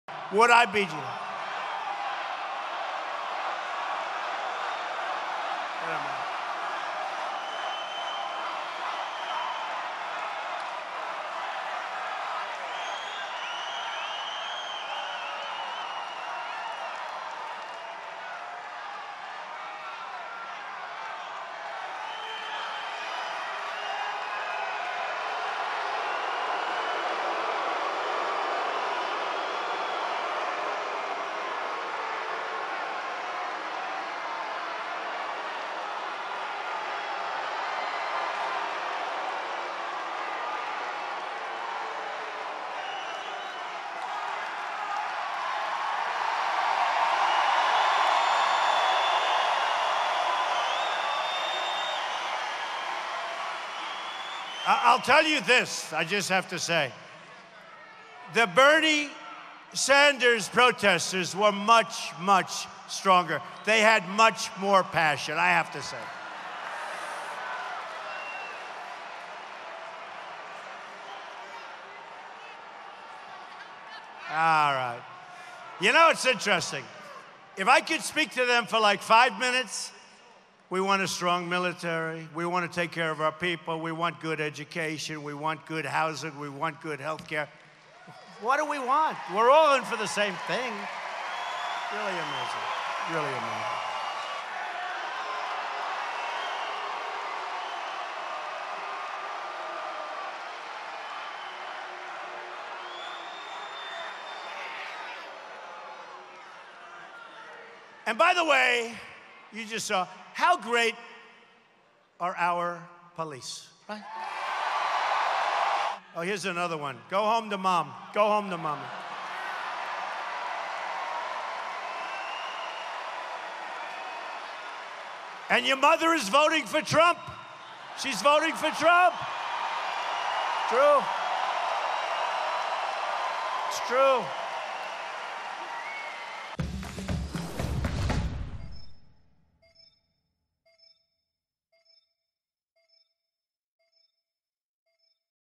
Trump Protesters Interrupt Donald Trump's Speech
Protesters Interrupt Donald Trump's Speech | Multiple protesters interrupted Donald Trump's speech while he was in Pennsylvania.